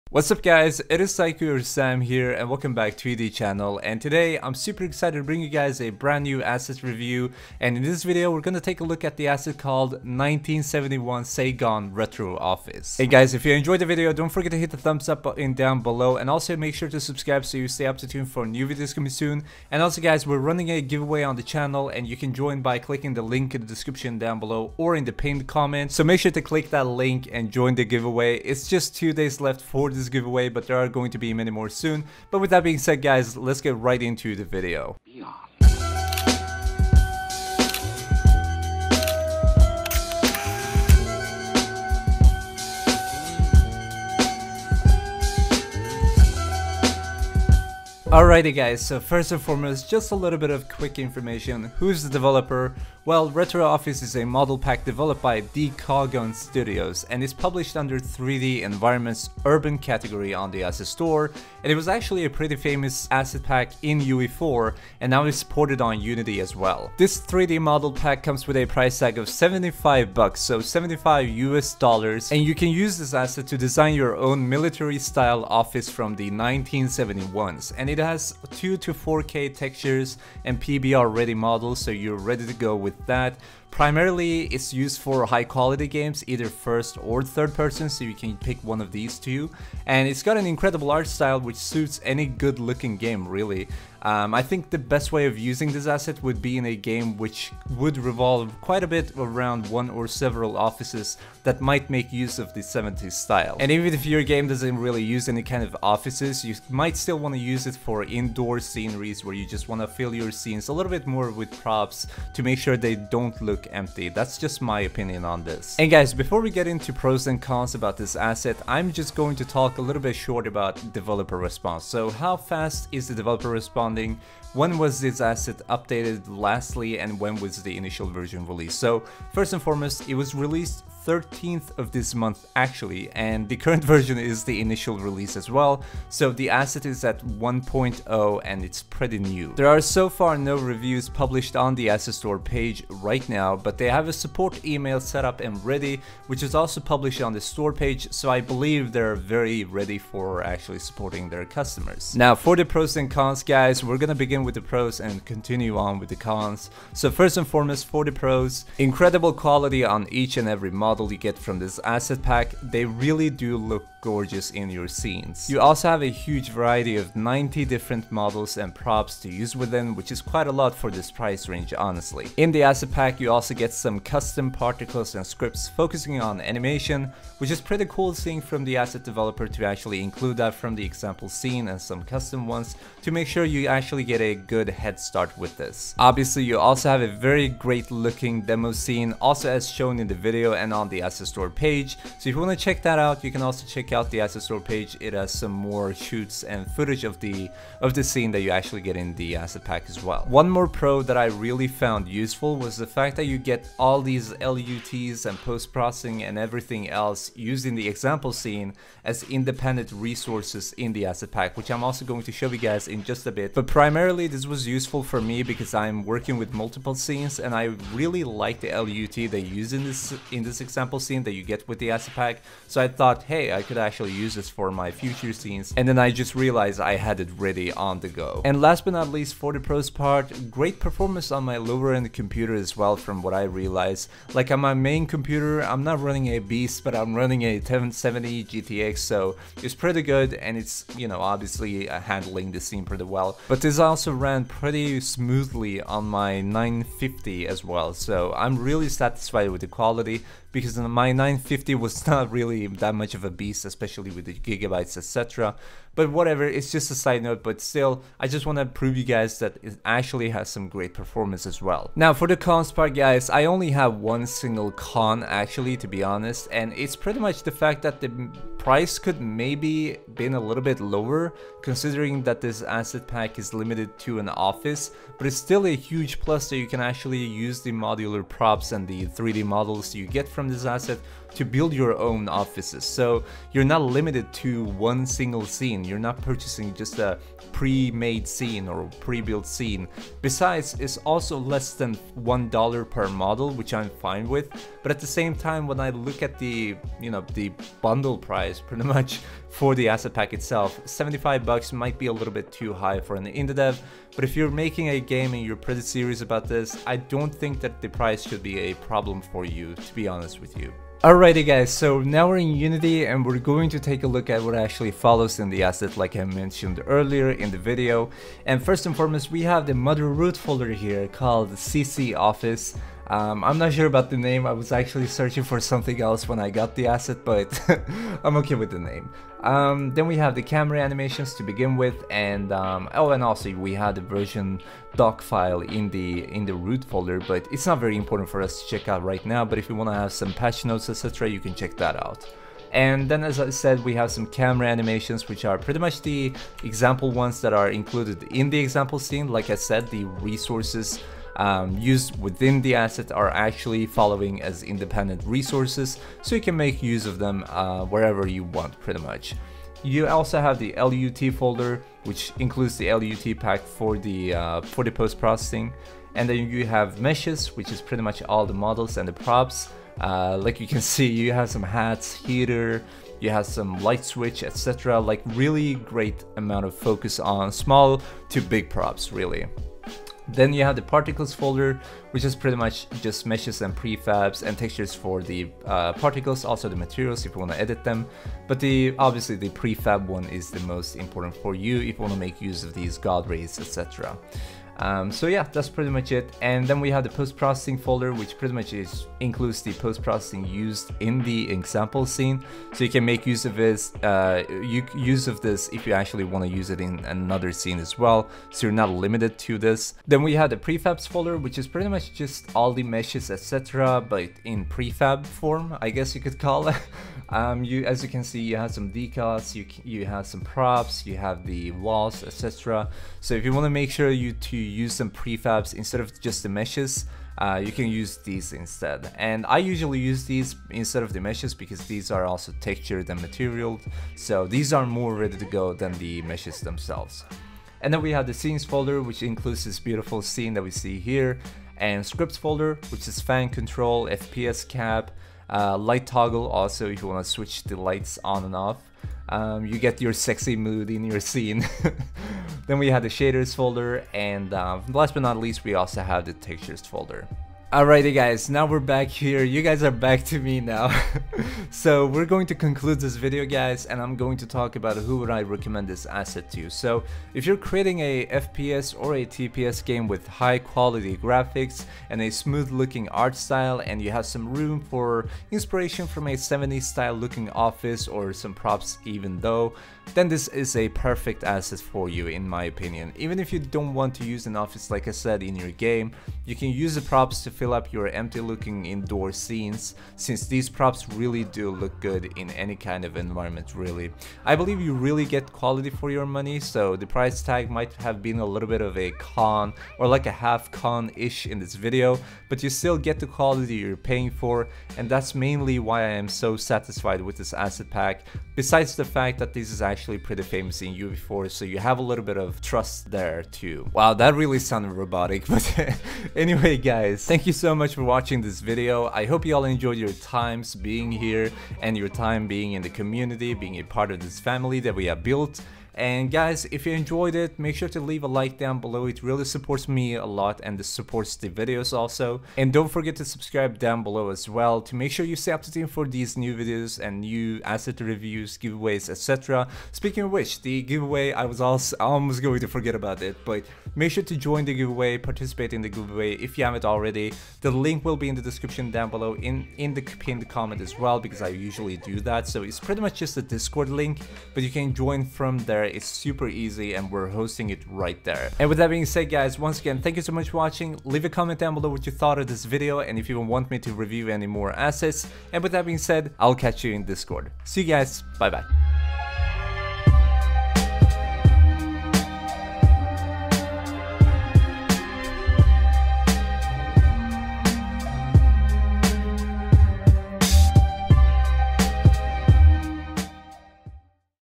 Asset Review: Retro Office 1971 | Unity 3D
♥ VIDEO LIKE GOAL:
• 105 Likes!

In this episode of Asset Reviews, we'll go through the asset 1971 Saigon - Retro Office!
Also, comment on what assets you want me to review.

Partnered by Curse, UnionForGamers.
Owner of MESVER & MESVER NETWORK